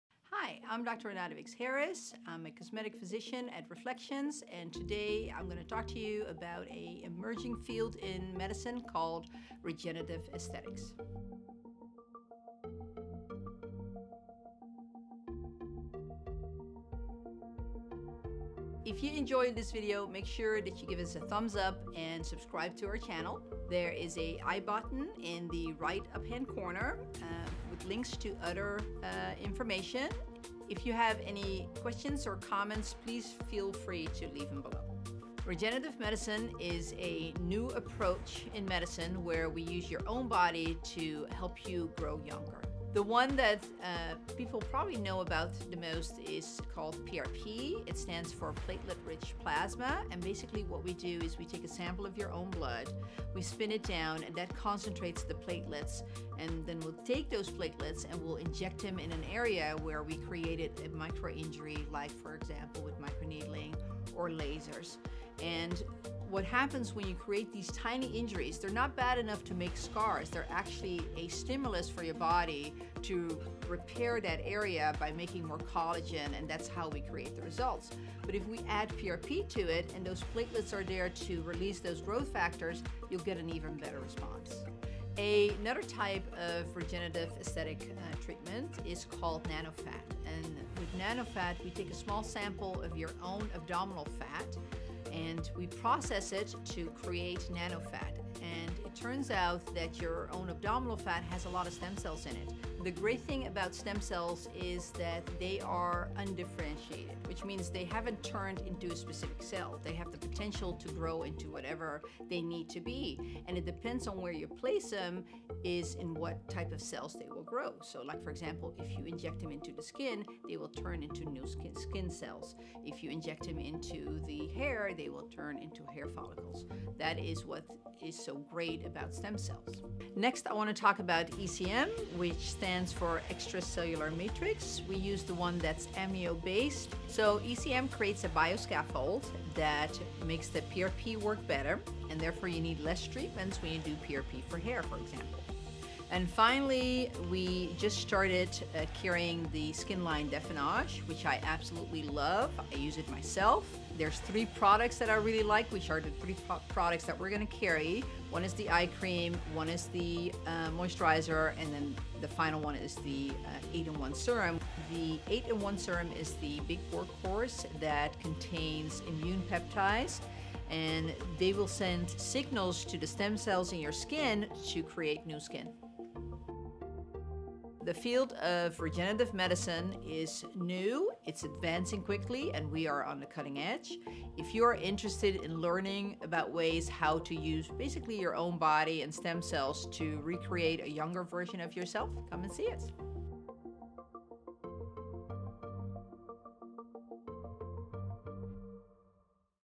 Regenerative Aesthetics [PRP, Stem Cells, & More!] Using Your Own StemCells for AntiAging
Regenerative Aesthetics refers to a category of cosmetic treatments that make use of your own body for anti-aging treatments. This includes using your own stem cells and blood (PRP), as well as sending bio-identical signaling to your cells to turn over at a faster rate, build new types of cells, etc.

In this video, Cosmetic Physician Dr. Renata Wix-Harris walks us through the main types of regenerative aesthetics treatments that have a strong enough body of scientific evidence behind them to meet our rigorous standards in our practice. She covers:

Nanofat (also called micronized fat or microfat) which makes use of the highest concentration of fat cells in adults, found within the fat of our midsection (tummy and love handles) to build new cells and restore areas that have been damaged or degraded by the aging process.

PRP (platelet-rich plasma) which makes use of the growth and healing factors within your own blood to speed recovery times and produce bigger results from paired treatments, such as microneedling paired with PRP.

Defanage skincare, a specially formulated skincare that sends signals to your cells to speed up turnover, causing the body to grow new skin cells and rejuvenate itself.